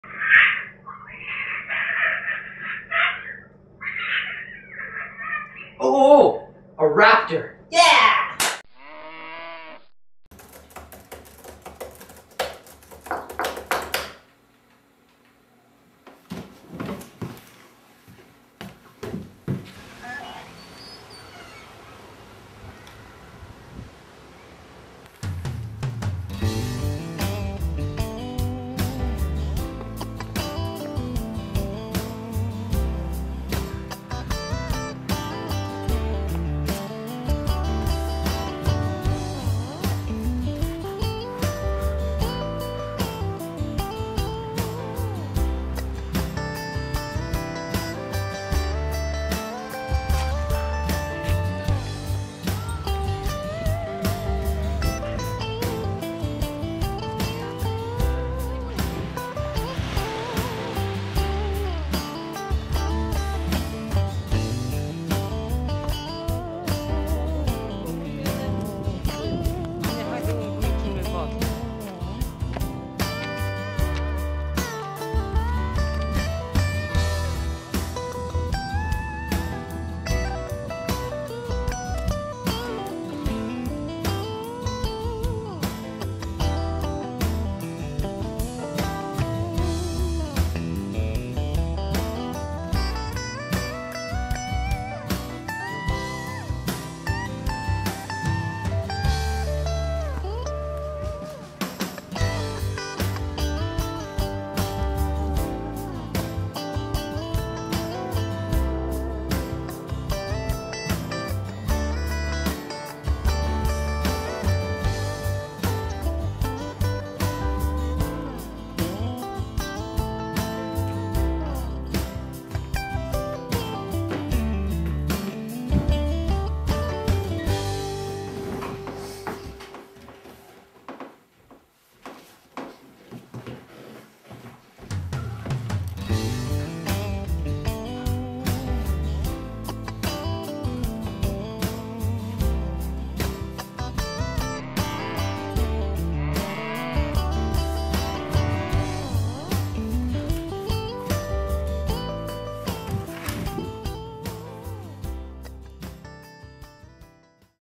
String Theory
An ordinary boy is sent on a journey of self-discovery by a piece of purple string.